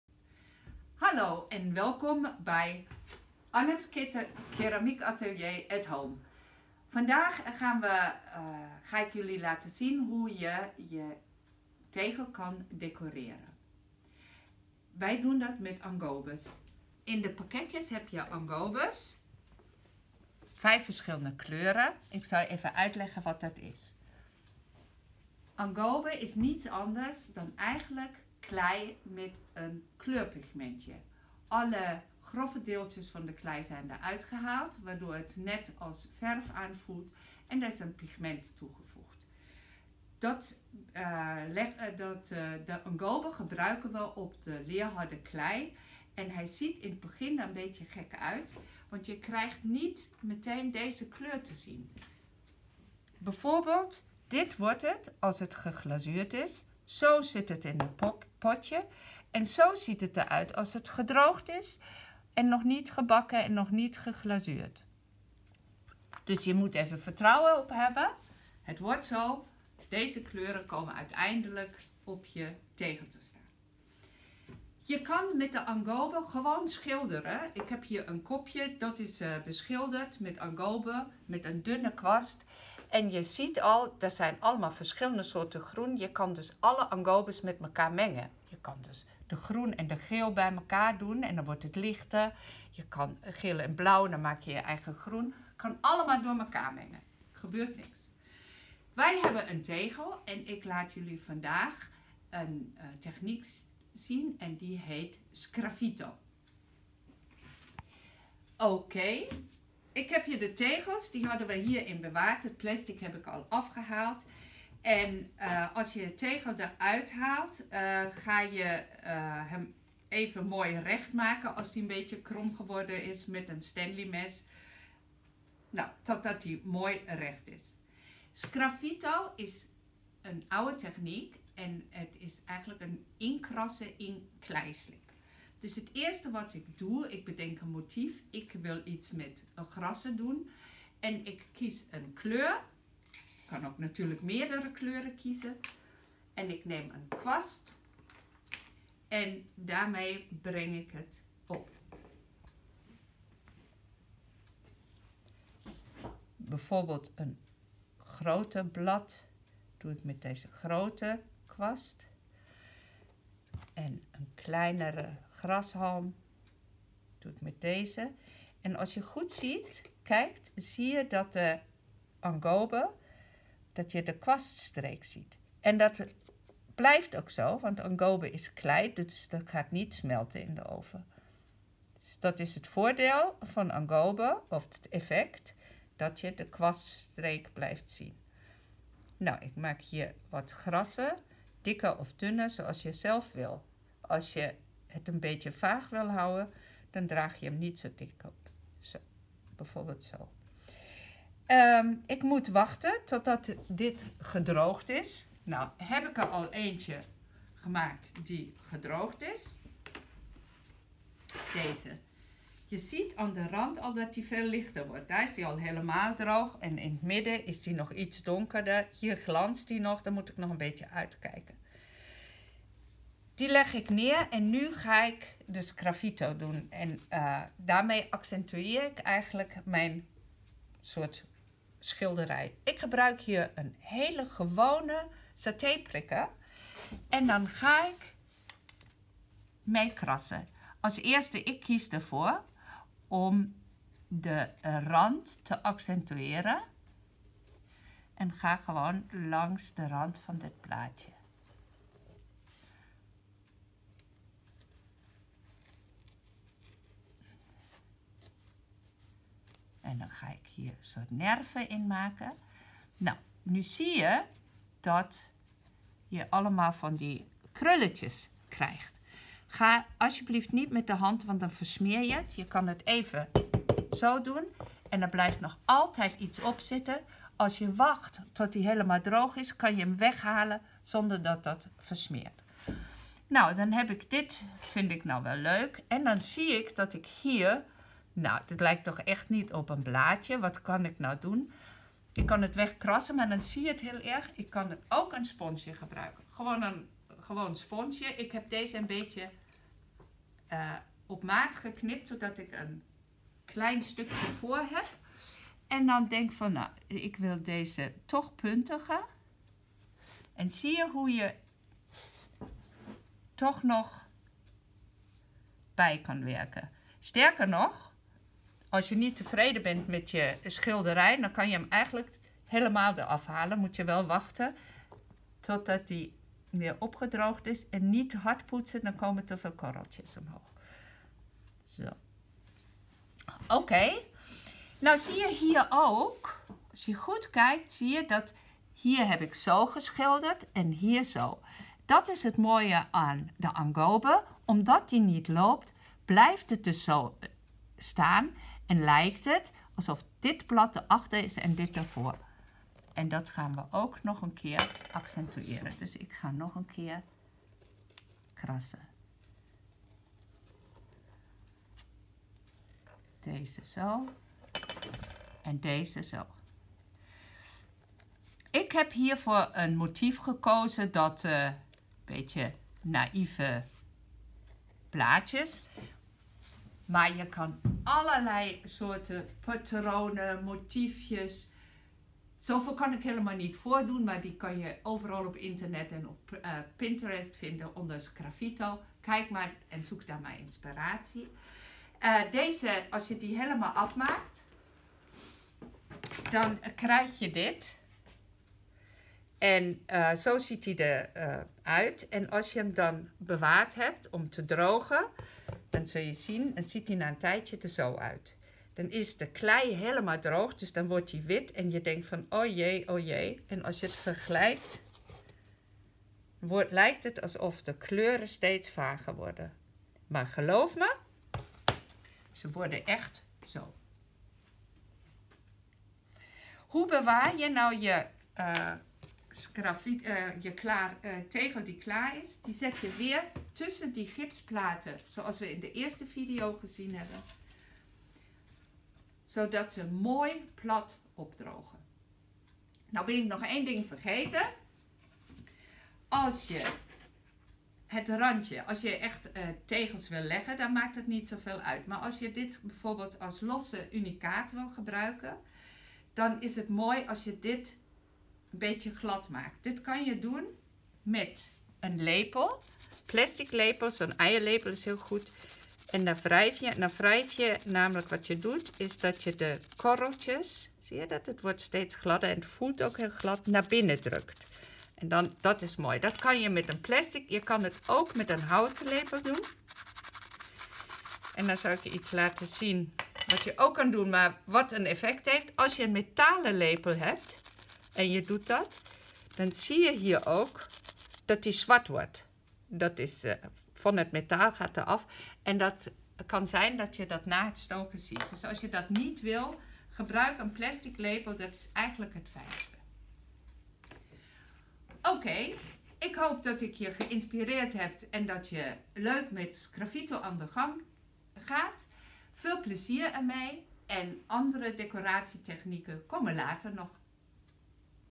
Decoreren met sgraffito (AKA
In deze video laat Guzi Nold Zien hoe je werken van ongebakken klei kunt decoreren. Bijvoorbeeld het schaaltje of de tegels uit een eerdere video.